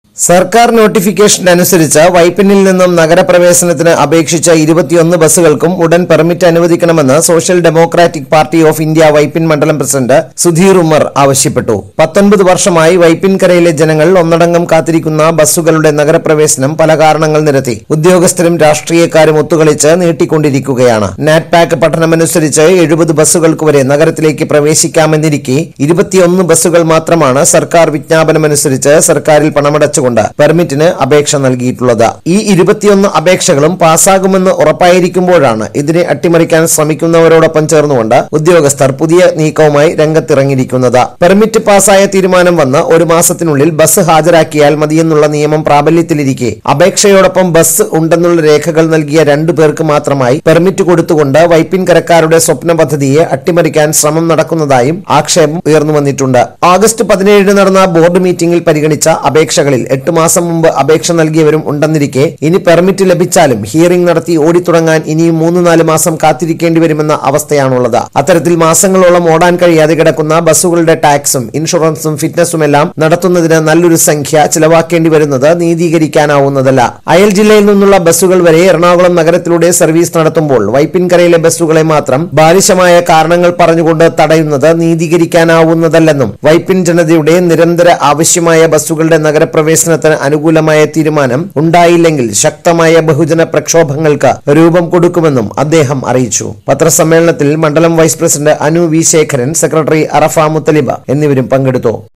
വൈപ്പിൻ കരയിൽ നിന്നും നഗര പ്രവേശനത്തിന് അപേക്ഷിച്ച എല്ലാ ബസുകൾക്കും പെർമിറ്റ് അനുവദിക്കണം-SDPI
സർക്കാർ നോട്ടിഫിക്കേഷൻ അനുസരിച്ച് വൈപ്പിനിൽ നിന്നും നഗര പ്രവേശനത്തിന്  അപേക്ഷിച്ച 21 ബസുകൾക്കും ഉടൻ പെർമിറ്റ്‌ അനുവദിക്കണമെന്ന് സോഷ്യൽ ഡെമോക്രാറ്റിക് പാർട്ടി ഓഫ് ഇന്ത്യ.